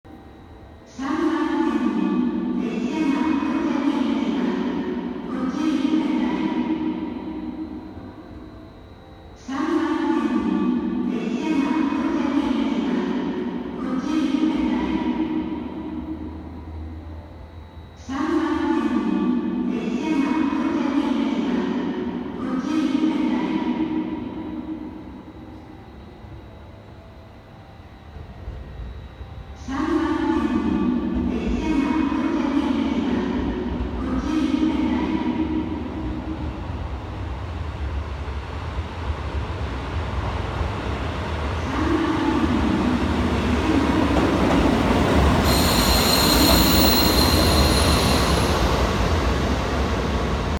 旭川駅に入線するキハ40系の単行「快速なよろ」
キハ40系の単行「快速なよろ」が旭川駅に入線するところです。近代的な旭川駅に国鉄風の気動車が入って来るシーンは、それはそれで面白いものです。